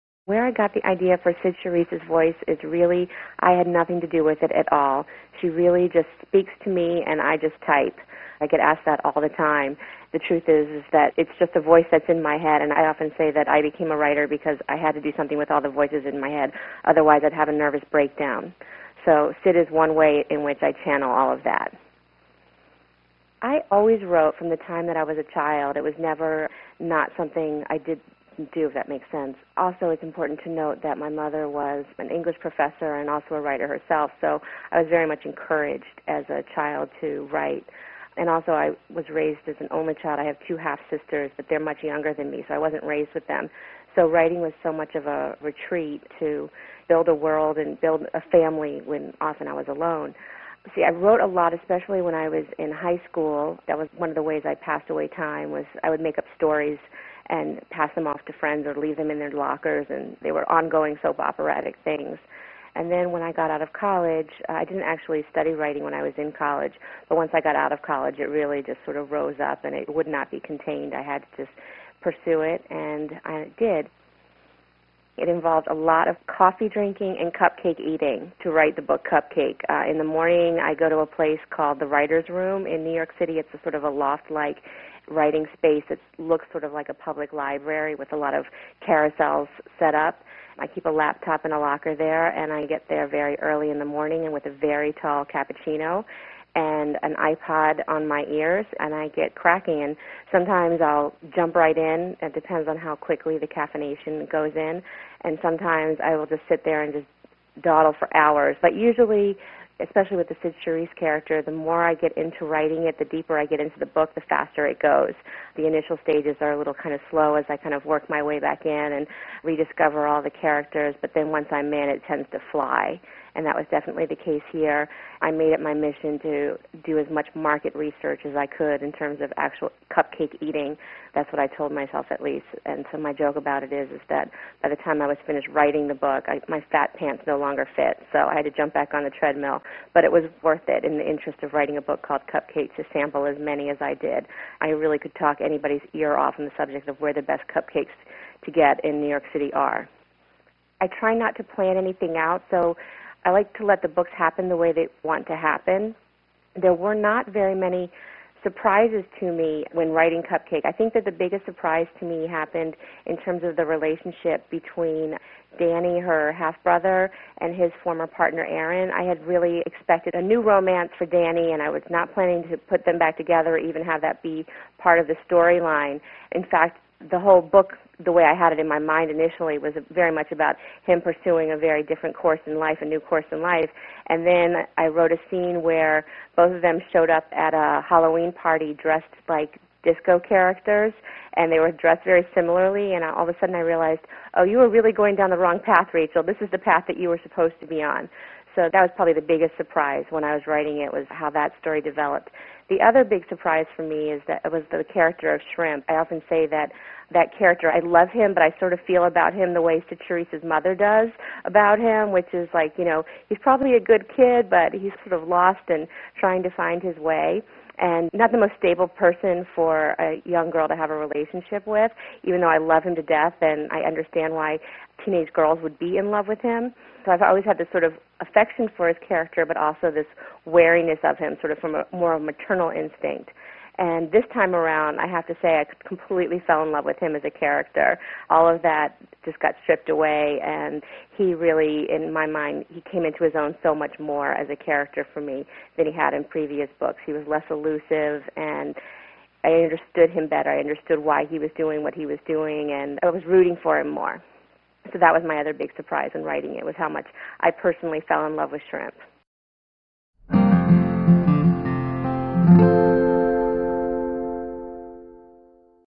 Rachel Cohn: Cupcake
In her third outing, reformed hellion Cyd Charisse moves from San Francisco to start a new life in New York City.